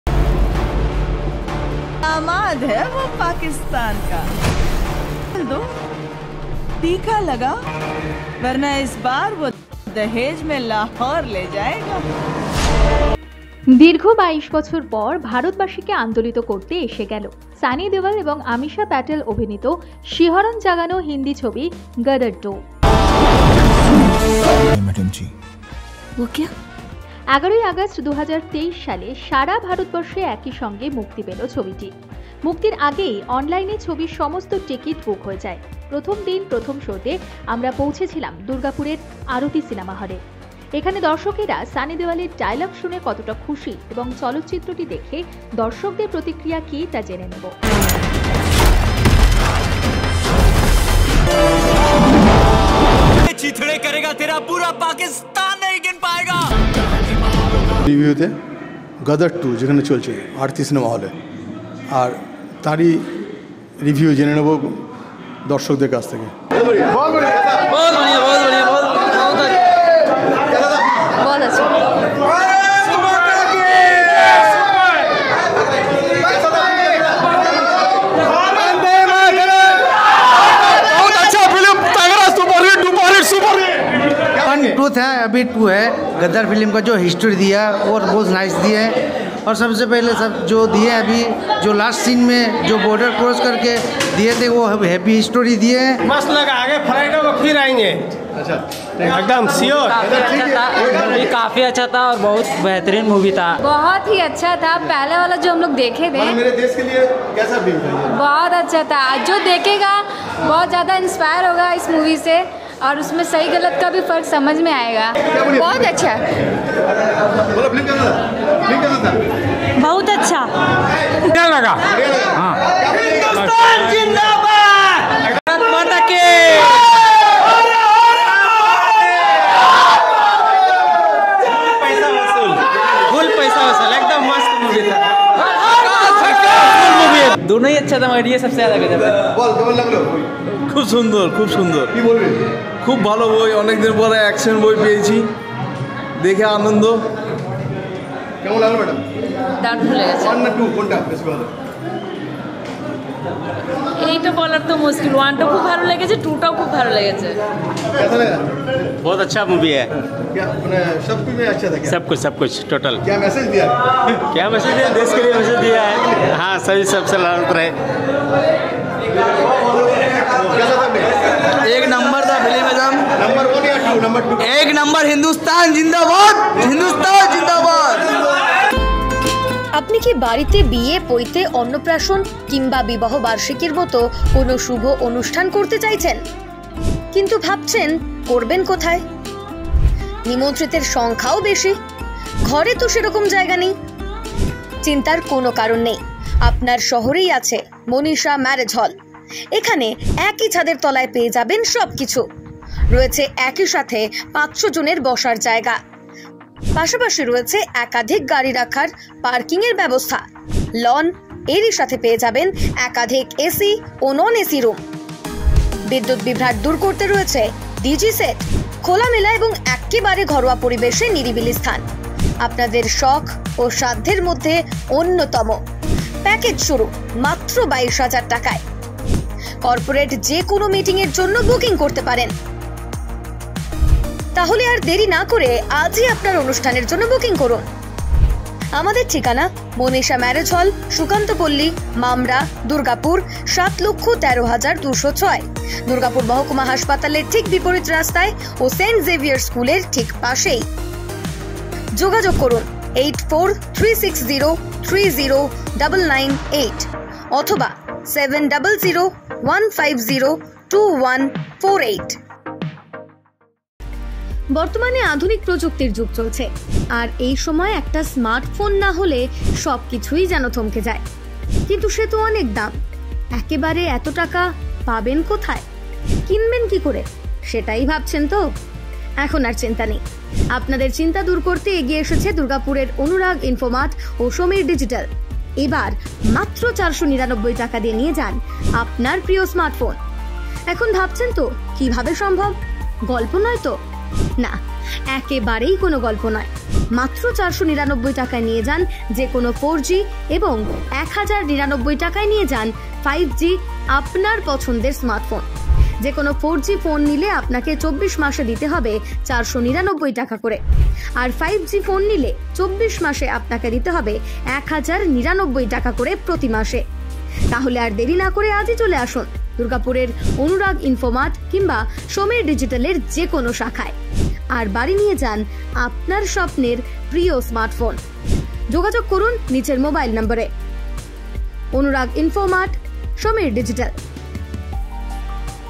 দীর্ঘ ২২ বছর পর স্বমহিমায় সানি দেওলের "গদর 2"
ছবি মুক্তির আগেই হাউসফুল টিকিট। আপ্লুত দর্শক